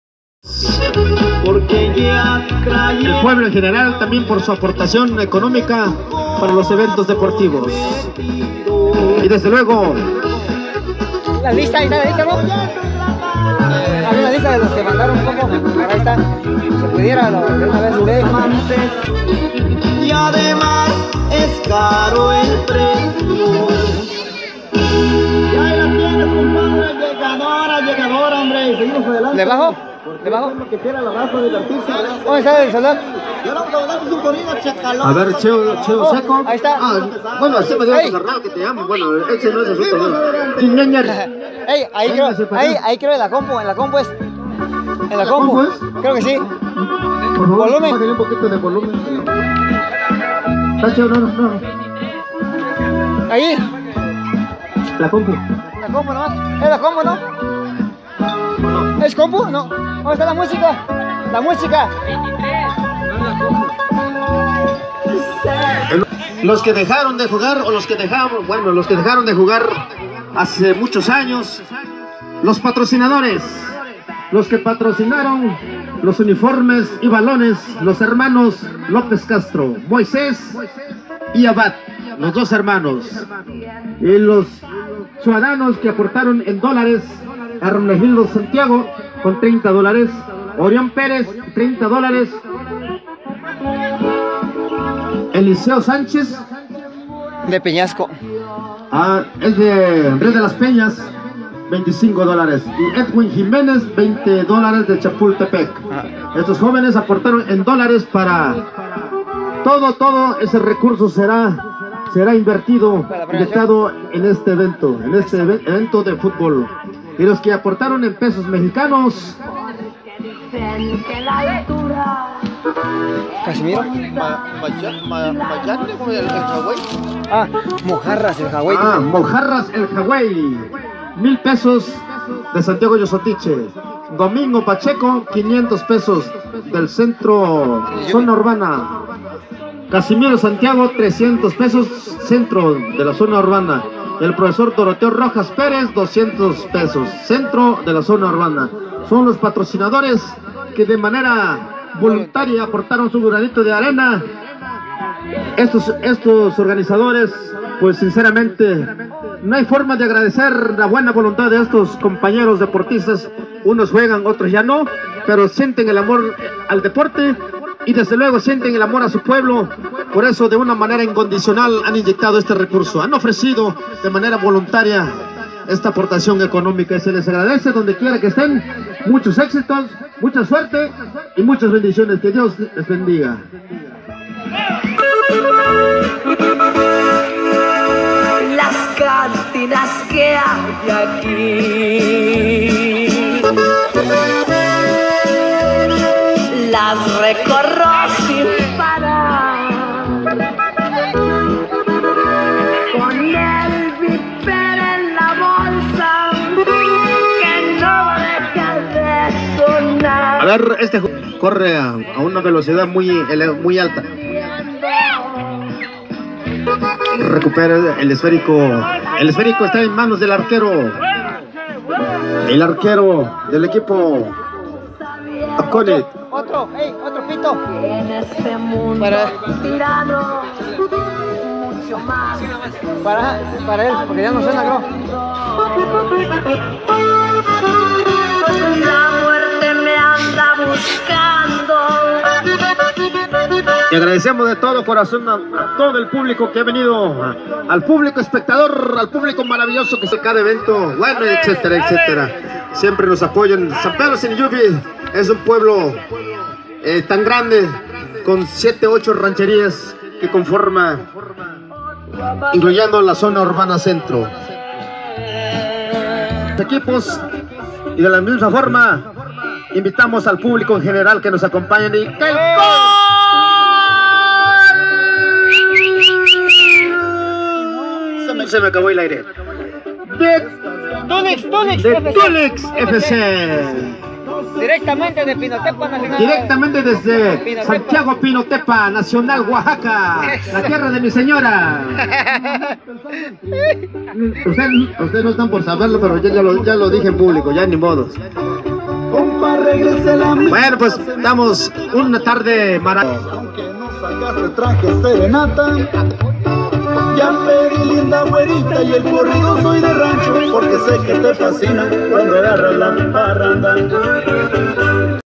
Futbol Siniyuvi 2018
Último partido del día 14 de Enero 2018
Tulix FC proveniente de la región de Pinotepa Nacional vs ACONIC de Zimatlán de Lázaro Cárdenas.